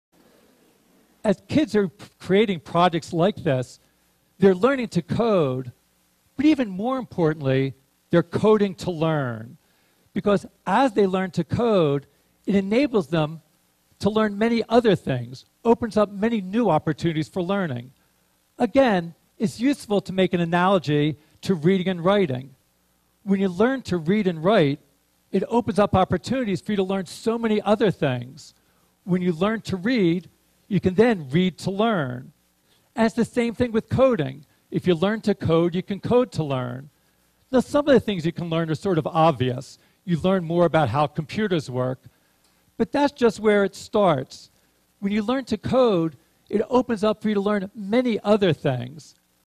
Mitch Resnick Coding to Learn Clip
We first learn to read and write. Then, we read and write to learn. According to Dr Mitch Resnick of MIT Lifelong Kindergarden, Coding is the same